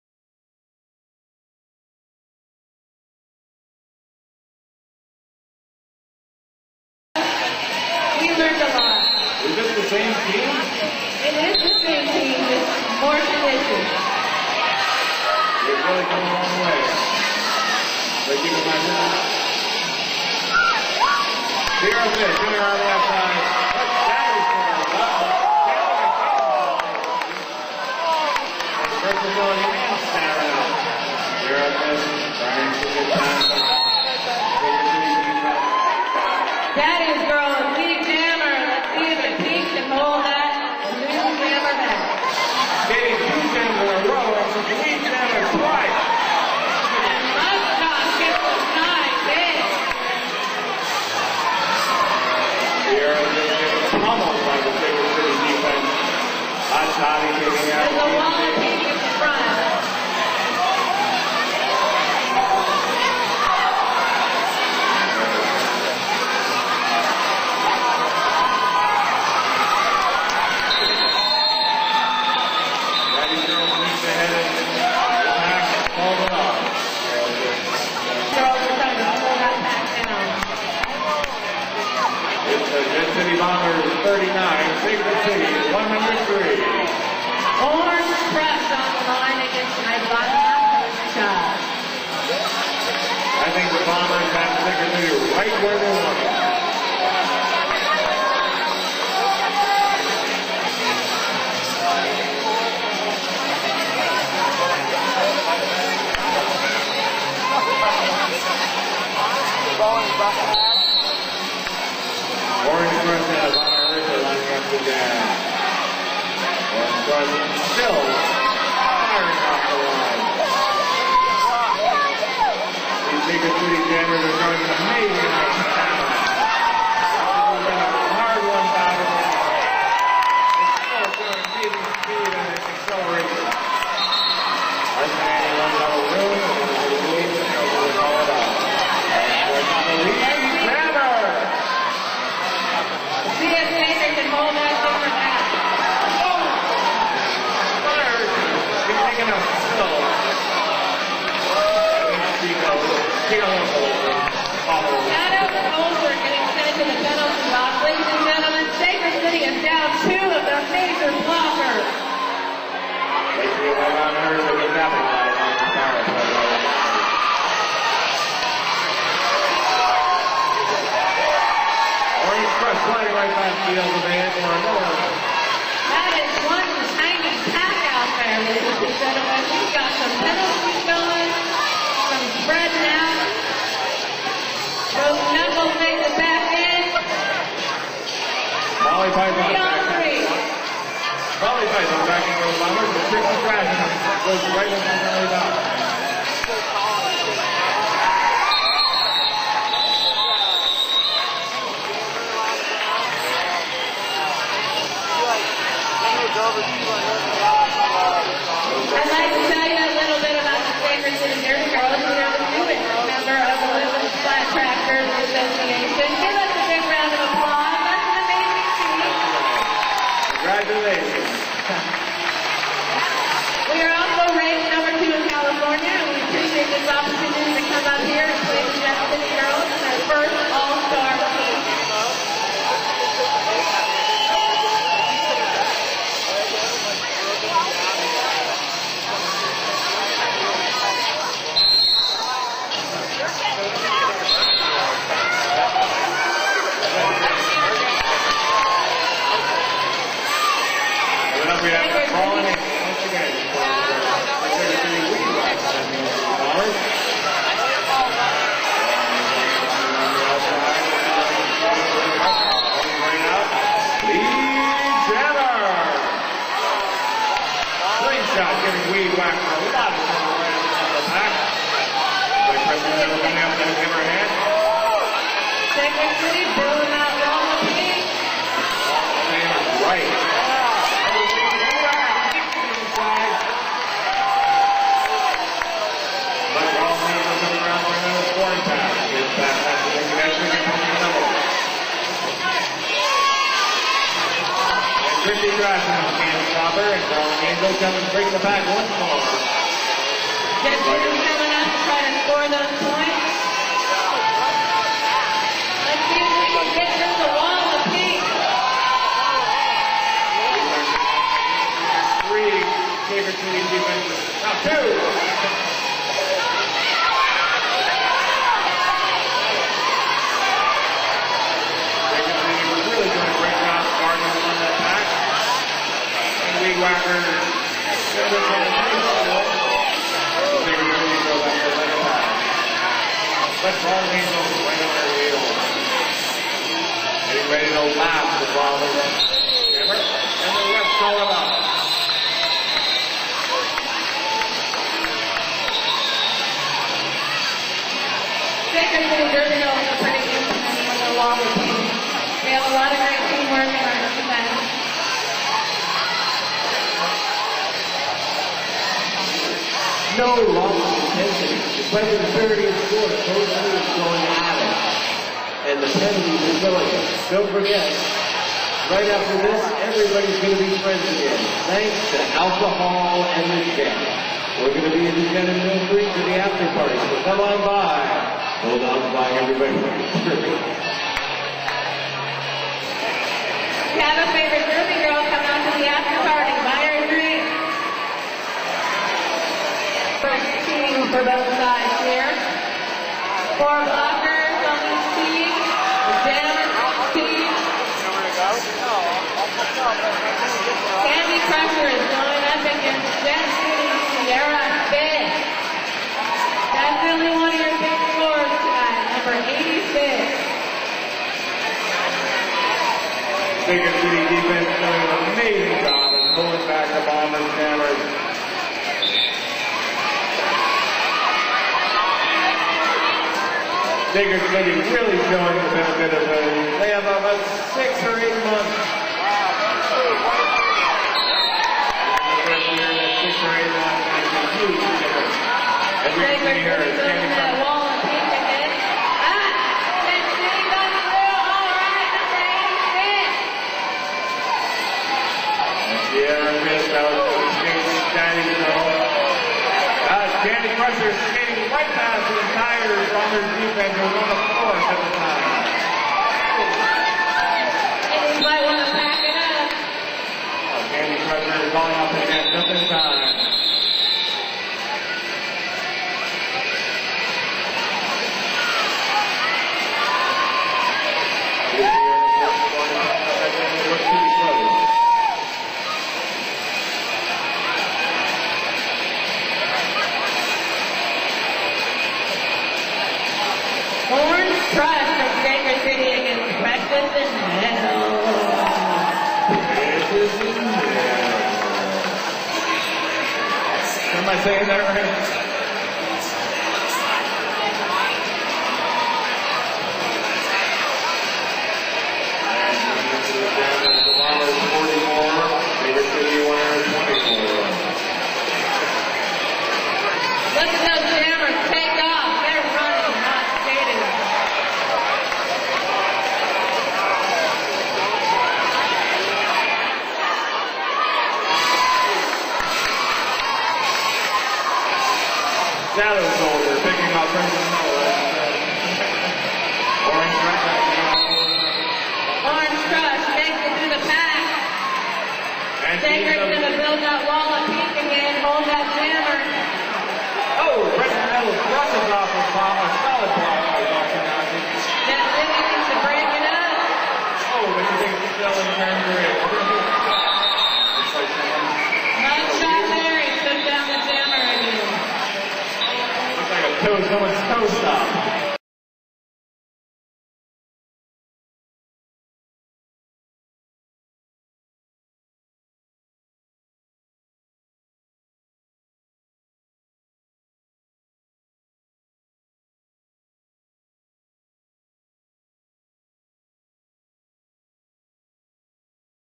Jet City Bombers - Sacred City 10/27/7 H2P3
The Jet City Rollergirls of Everett Washington have had two exhibition bouts.  They selected their travel team, the Bombers, and instead of taking on a local league of similar experience they decided to invite Sacred City of Sacramento for their first interleague competition.  Quite courageous.
Half Two - Part Three